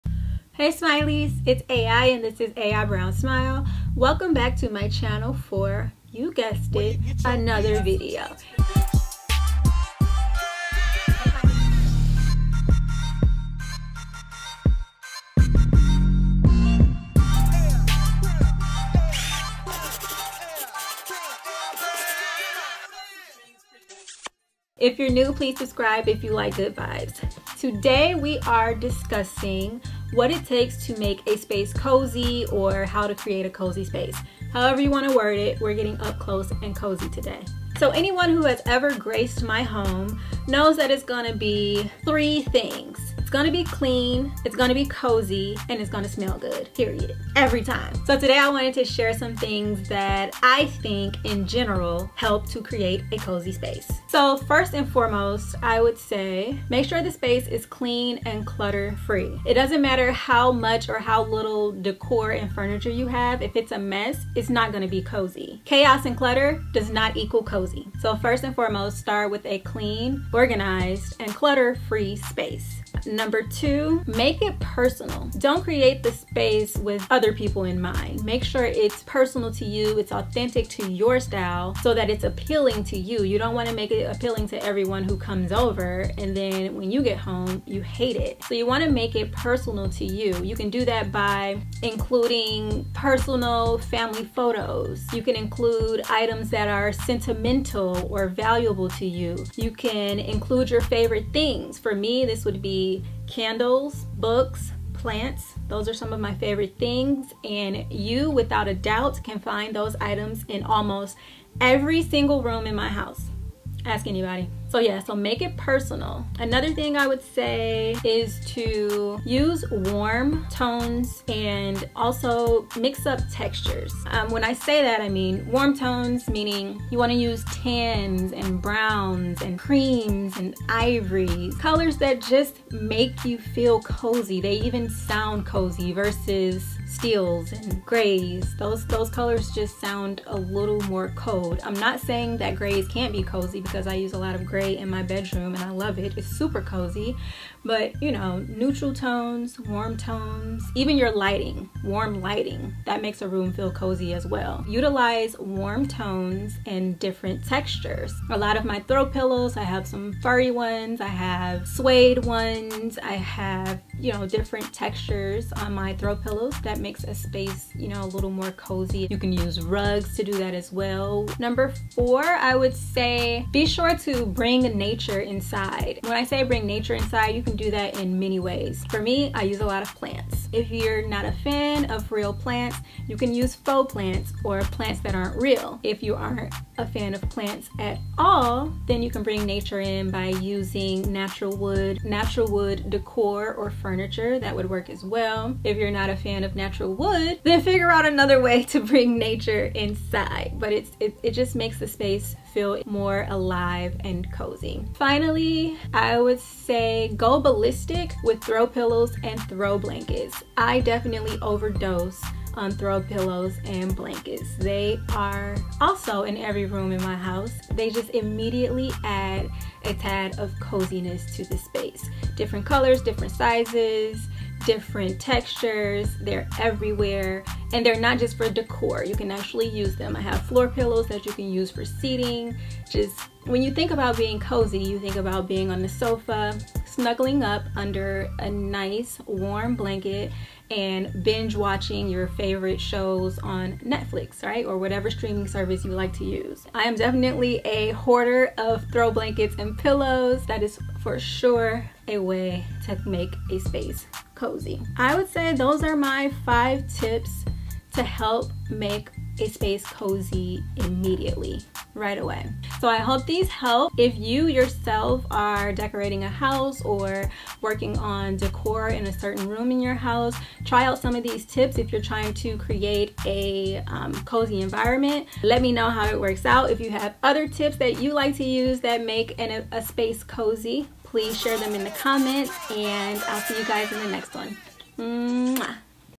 aibrownsmile | How To Make Any Room COZY! 5 EASY Tips for Creating a Warm and Inviting Space
Hey Smileys hey!! I have been on a mission to make our house feel like home since the moment we signed the dotted line. My goal was to create spaces that were warm, inviting and super COZY! There are many ways to make a room feel cozy and comfortable. The tips shared in this video are a few of my go-to, tried-and-true, ways to create a cozy space.

XoXo,
AI

Spread L O V E.
Promote P E A C E.
Cultivate P O S I T I V I T Y.
Find the B E A U T Y in all things.

smil·ey
/ˈsmīlē/

@Hustle Dream Production